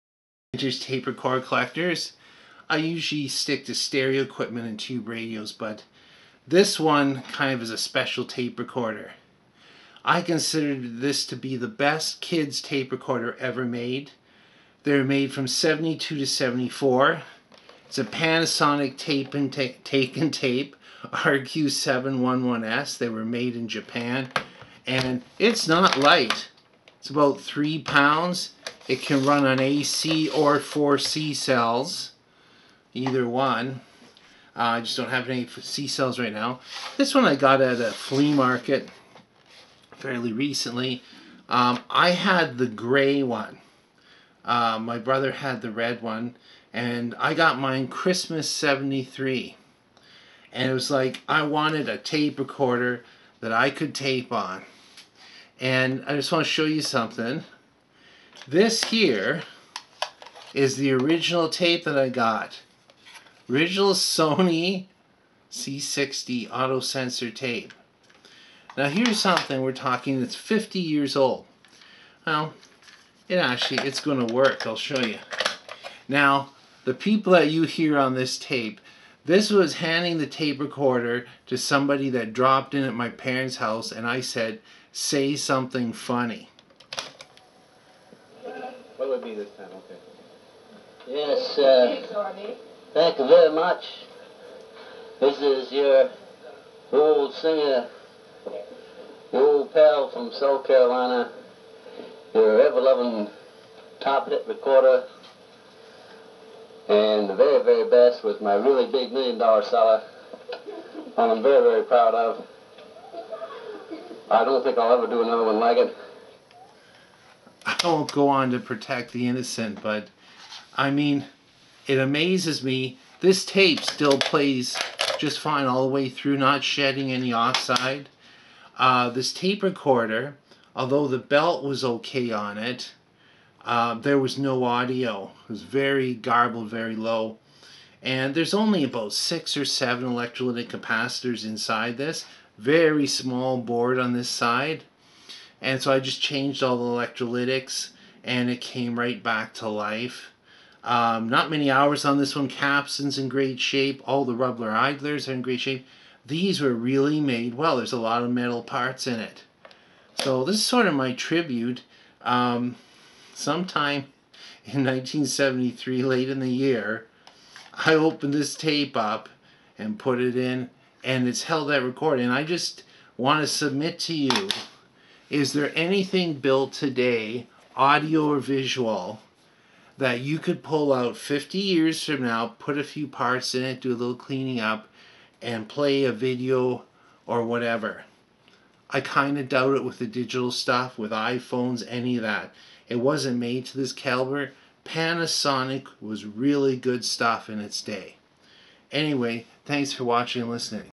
Panasonic Take n' Tape RQ-711s plays tape from 1973!
I recorded this tape on a Take n' Tape in Dec 1973 and it still works.